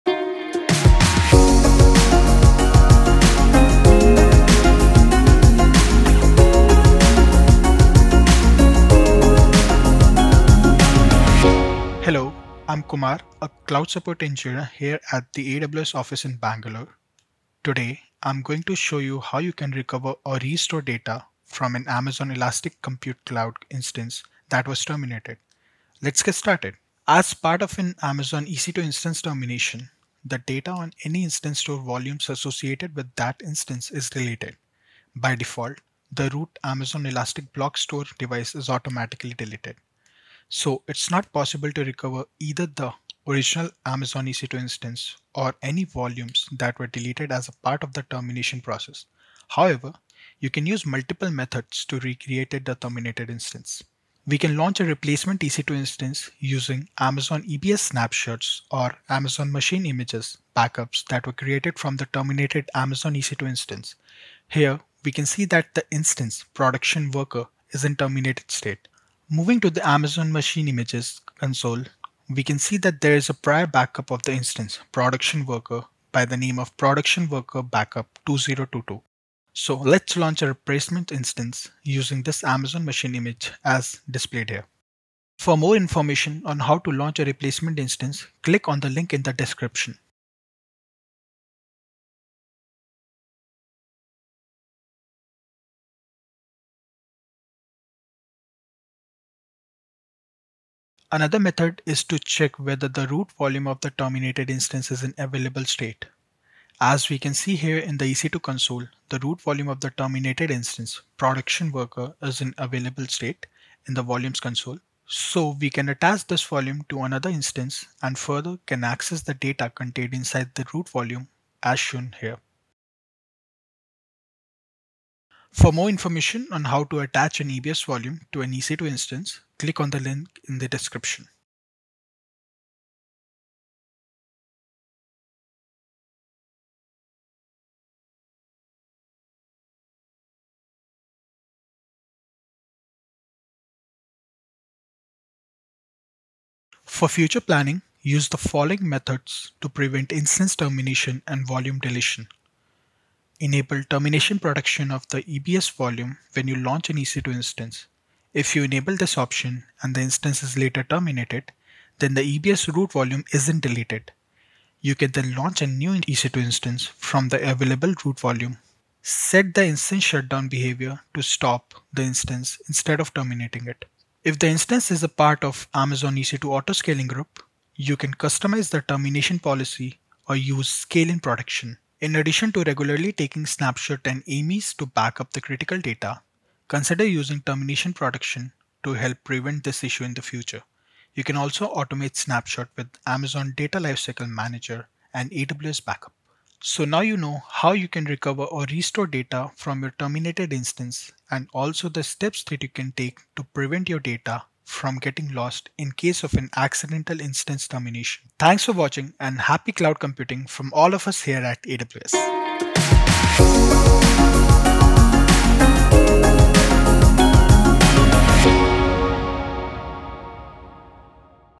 How do I re-create a terminated EC2 instance?
For more details see the Knowledge Center article with this video

Kumar shows you how to re-create a terminated EC2 instance.

0:00 Intro
0:28 Starting notes
0:56 Launch replacement EC2
1:45 Check of Root is in available state
2:11 Mount in Terminal
2:27 Ending Notes
3:17 Ending